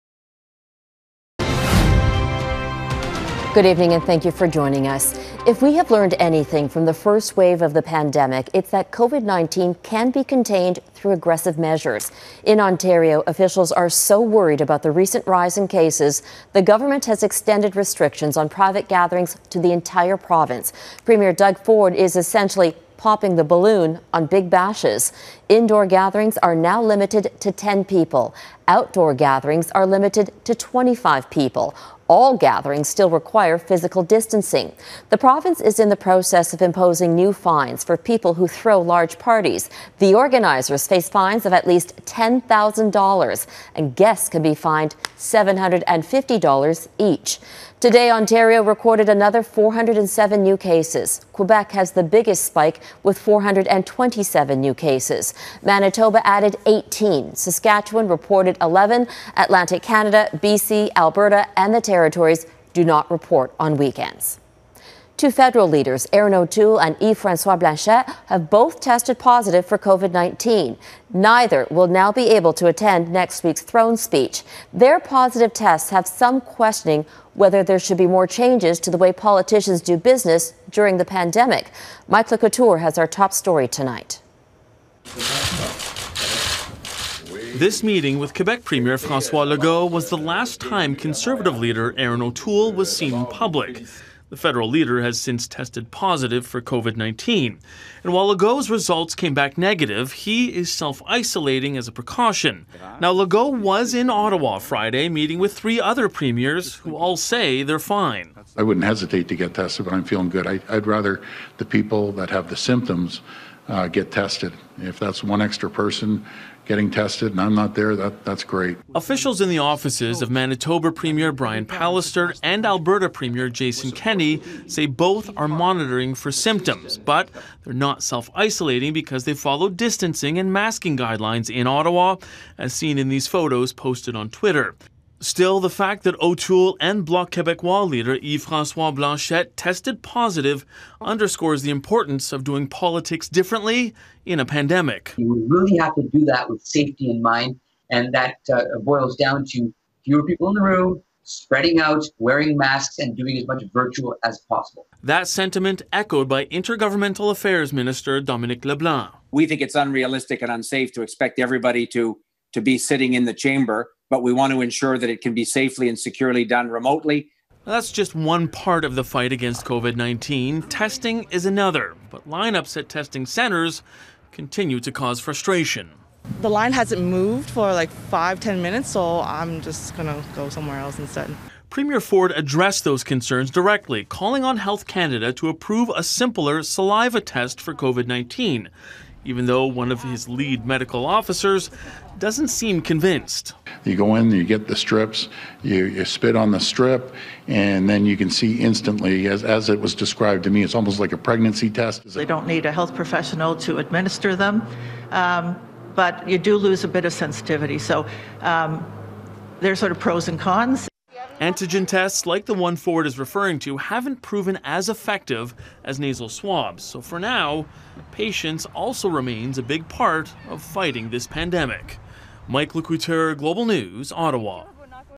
Coronavirus: Two Canadian political leaders contract COVID-19
Two Canadian federal party leaders, Conservative Erin O'Toole and Bloc Québécois Yves-François Blanchet, have tested positive for COVID-19. Neither will be able to attend the Throne Speech when Parliament resumes on September 23. As Mike Le Couteur explains, that's raising questions over the business of politics during a global pandemic